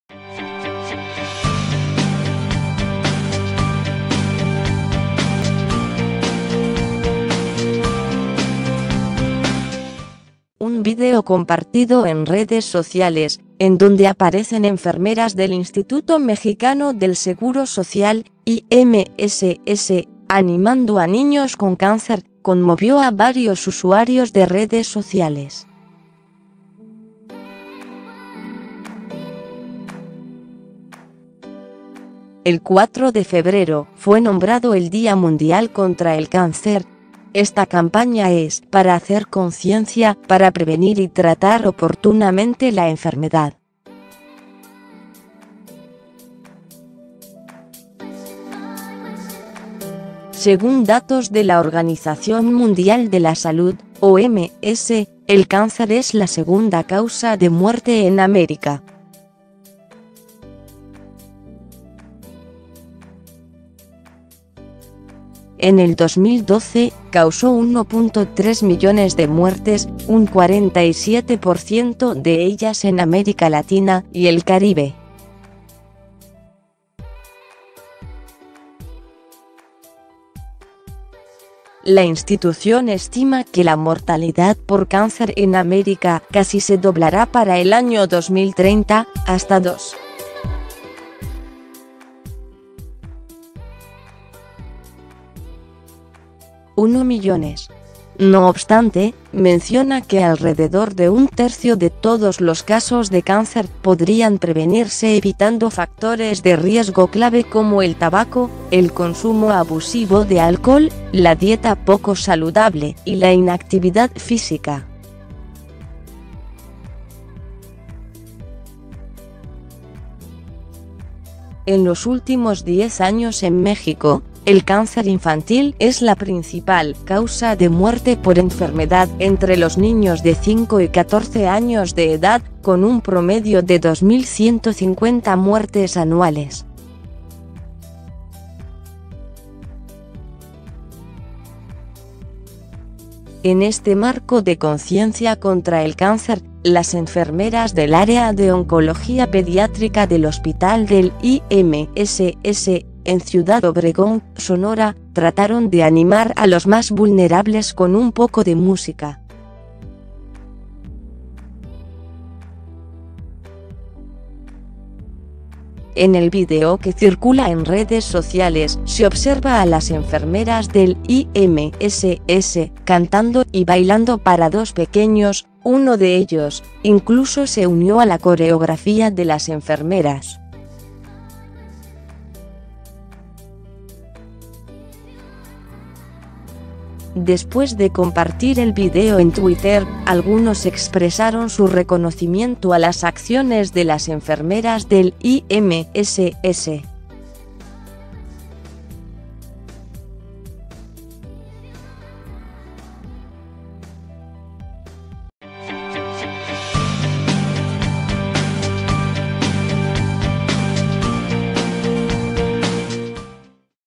Conmueven enfermeras del IMSS al animar a niños con cáncer
Fuente: goo.gl/ePtZew